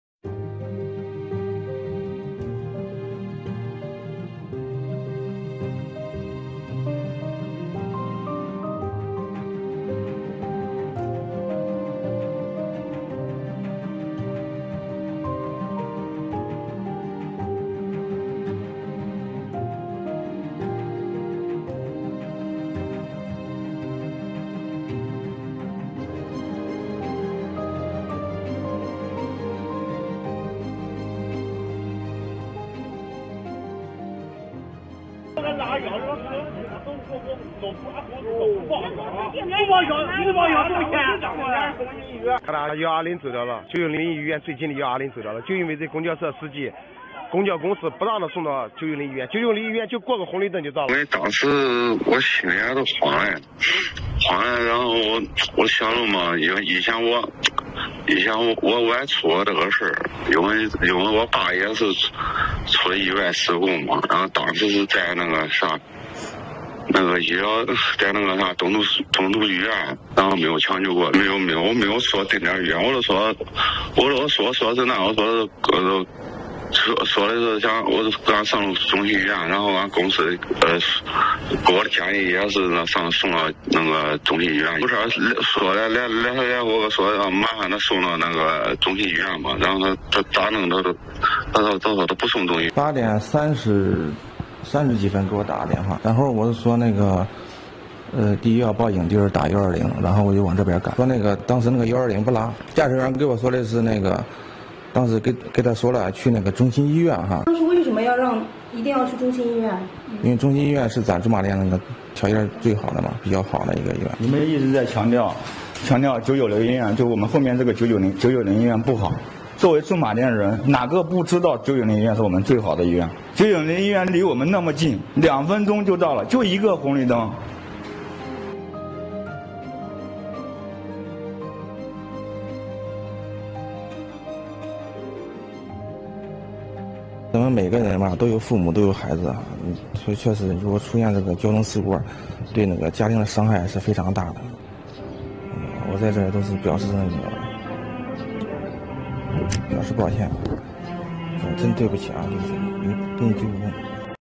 河南一公交司机肇事撞人后拒让120载走：被撞者不幸身亡
河南驻马店。杜先生反映，公交司机肇事撞父亲后，拒绝让120接走送至就近医院，坚持要送往中心医院。最终，杜先生的父亲被送往中心医院，但因失血过多等原因不幸离世。杜先生家人怀疑公交司机这样做，是因为中心医院是他们的定点医院，可以报销。司机赵某认为中心医院是当地最好。公交安全员道歉。